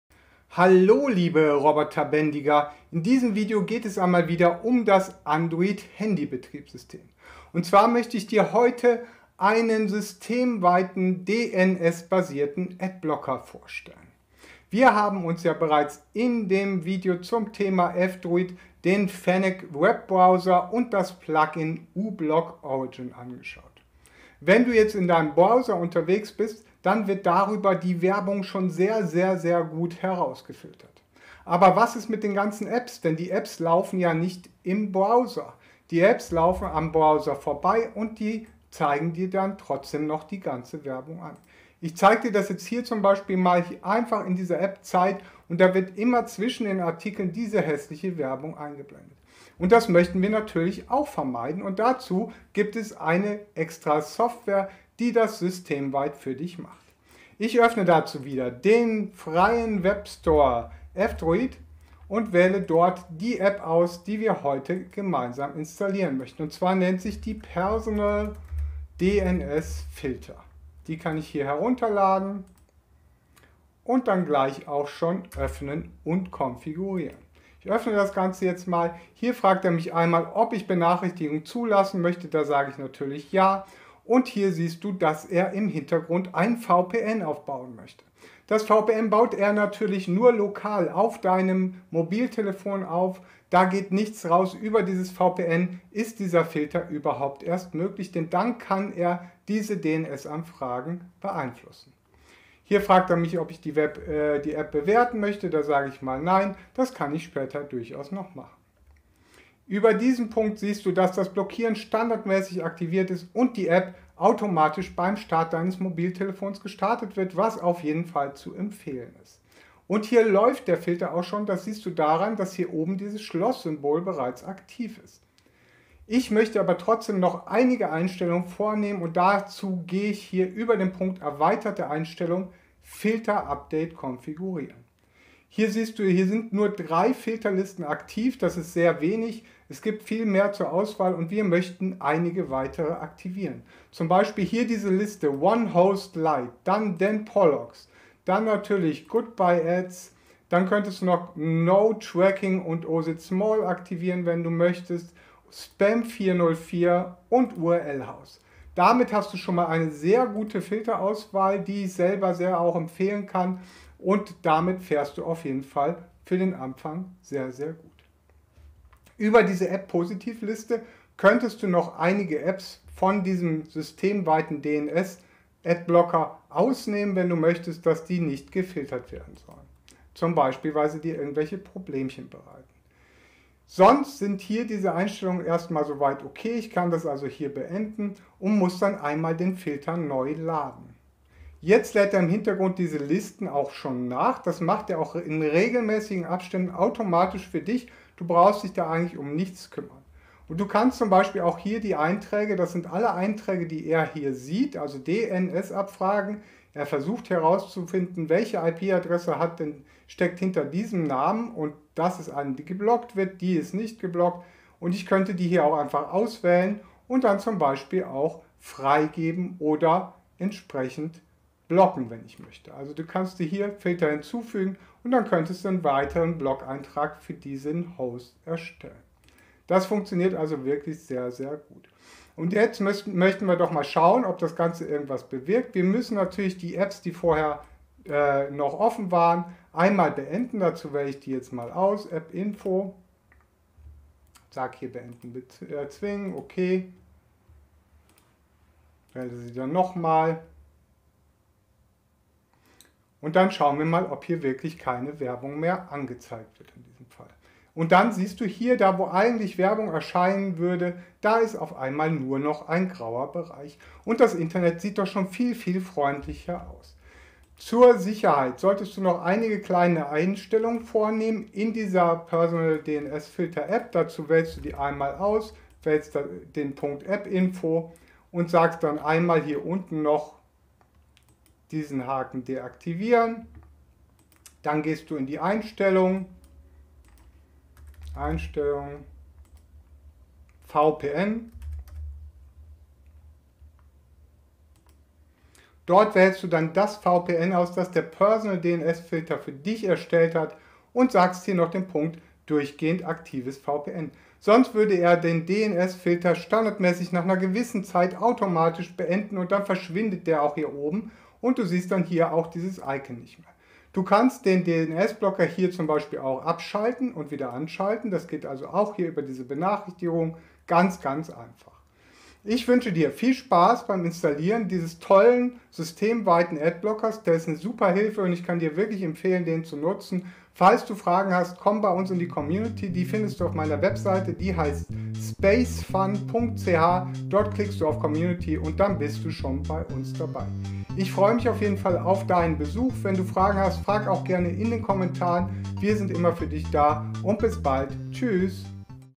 personalDNSfilter - dein systemweiter AdBlocker für Android
In diesem Video erkläre ich dir Schritt für Schritt, wie du auf deinem Android-Phone den systemweiten AdBlocker personalDNSfilter aus dem F-Droid Store installierst und einrichtest.

Ich zeige dir die optimalen Filtereinstellungen, damit du das Beste aus der App herausholen kannst. Außerdem teile ich einige wichtige Tricks und Tipps, um die App sicher und effektiv zu nutzen.

Egal, ob du Werbung auf deinem Smartphone blockieren oder deine Privatsphäre im Internet schützen möchtest – mit personalDNSfilter hast du die Kontrolle über deine Online-Erfahrung.

Viel Spaß beim Anschauen und Ausprobieren!